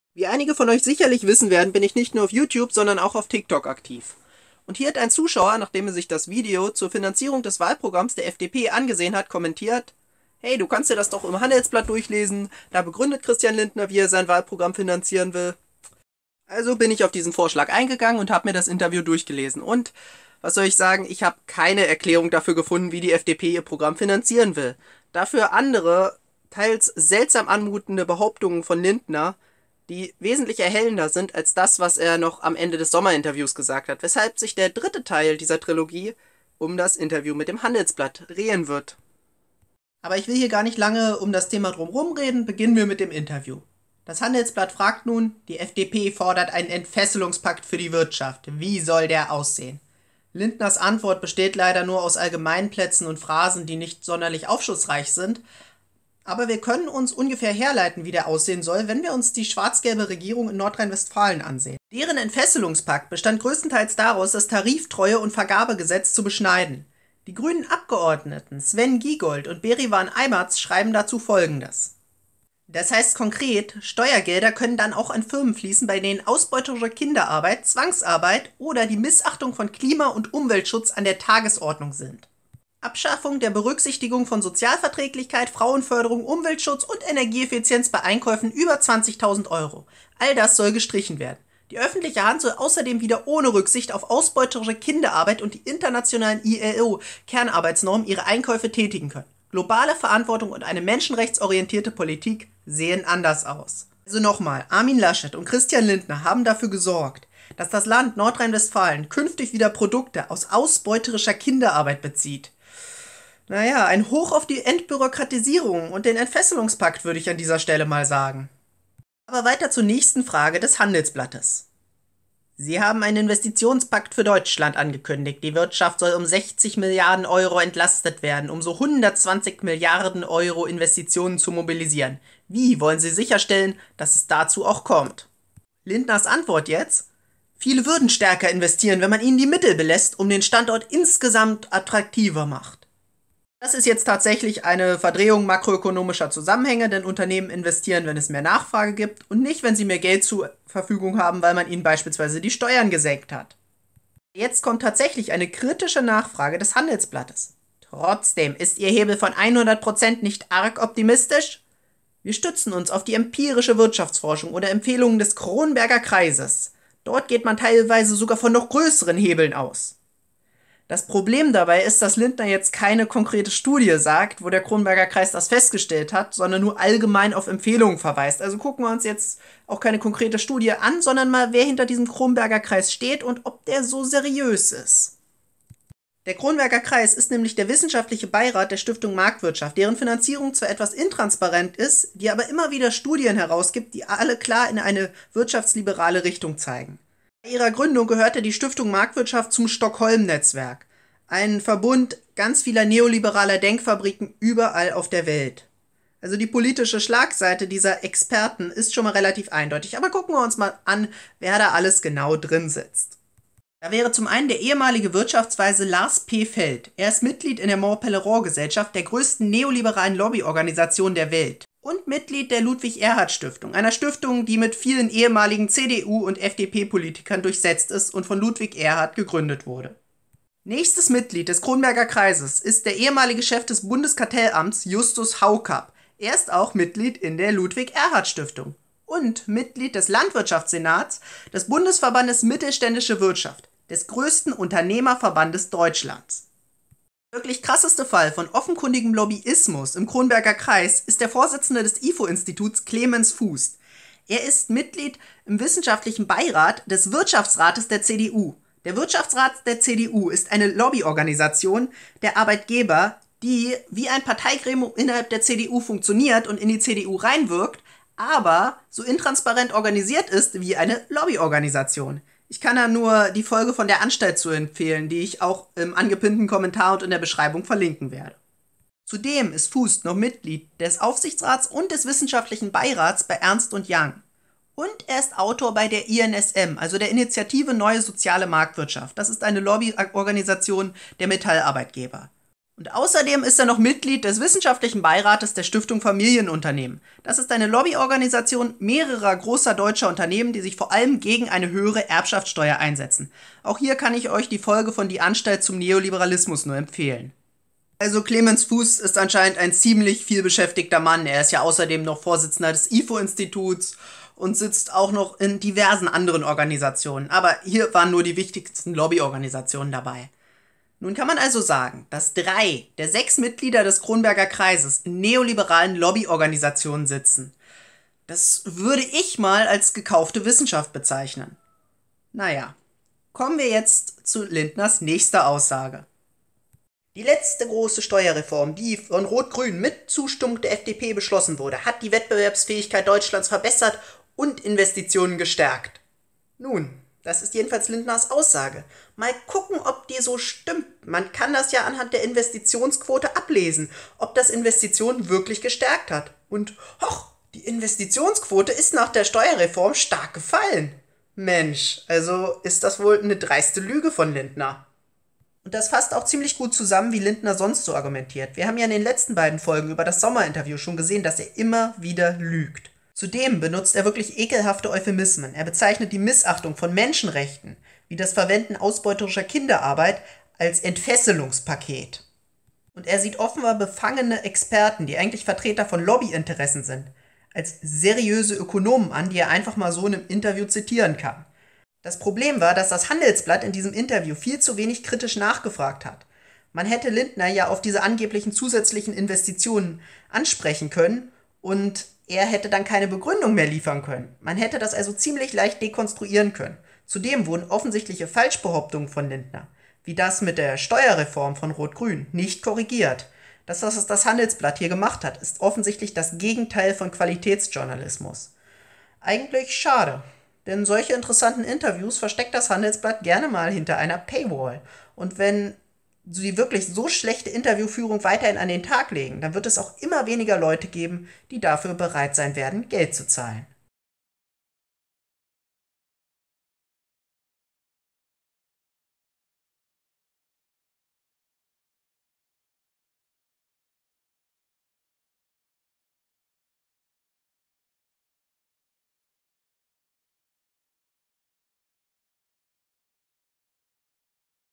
FDP-Chef Christan Lindner blamiert sich im Handelsblatt Interview |Teil 3: Lobbyisten = Experten
Der Vorsitzende der FDP Cristian Lindner zeigt im Interview mit dem Handelsblatt, dass er absolut keine Ahnung von Wirtschaftspolitik von der Finanzierung des FDP-Wahlprogramms  für die Bundestagswahl 2021 hat.

Kapitel
0:00 I. Schwarzgelb - Enffesselung vs. Menschenrechte
1:58 II. Der Kronberger Kreis
5:34 III. Lindner lügt schon wieder

Im 3. Teil geht es um die Begründung der Wirtschaftspolitik der FDP (Cristian Lindner will ja Finanzminister werden), die den Interessen von Reichen, Unternehmen und Superreichen dient. Cristian Lindner sagt dazu auch im Interview mit dem Handelsblatt, dass er sich auf den Kronberger Kreis beruft.

Die FDP möchte laut ihrem Wahlprogramm nach der Bundestagswahl 2021 die Steuern um 88 Milliarden Euro senken, wovon 76 Milliarden nicht gegenfinanziert sind. Der Spitzenkandidat der FDP Cristian Lindner weicht in diesem Handelsblatt Interview Fragen nach der Wahlprogramm Finanzierung konsequent aus. Er behauptet stattdessen, dass Steuersenkungen für Reiche und Unternehmen zu mehr Investitionen und mehr Arbeitsplätzen führen würden. Cristian Lindner agitiert zudem gegen Armin Laschet, Friedrich Merz, die CDU, die SPD und die Grünen weil sie die Schuldenbremse aufweichen wollten. Vom Spitzenkandidaten der FDP Cristian Lindner wird zudem behauptet, dass wir hier in Deutschland in einem Land mit Steuern leben würden.

Um es klar zu sagen: Die FDP stellt ihre neoliberale Ideologie vor wissenschaftliche Erkenntnisse der Volkswirtschaftslehre, um die Steuern für Reiche und Unternehmen zu senken.

Quellen (auch Teil 1+2)

Petition von Giegold und Aymaz an Armin Laschet und Cristian Lindner